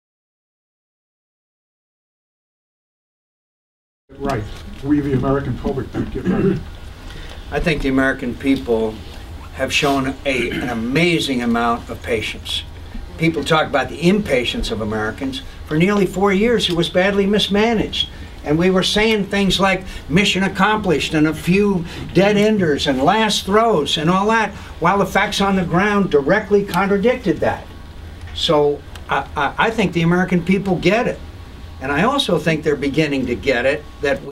John McCain: House Party 12/30/07
John McCain
House Party
Lebanon, NH 12/29/07

TRANSCRIPT:
MCCAIN: "I think the American people have shown an amazing amount of patience. People talk about the impatience of Americans. For nearly four years it was badly mismanaged.

"And we we're saying things like 'mission: accomplished' and 'a few dead-enders,' and 'last throes,' and all that while the facts on the ground directly contradicted that. So I think the American people get it."

LABEL: JM NH 12-30 (BH#48) AKHDF-ClipB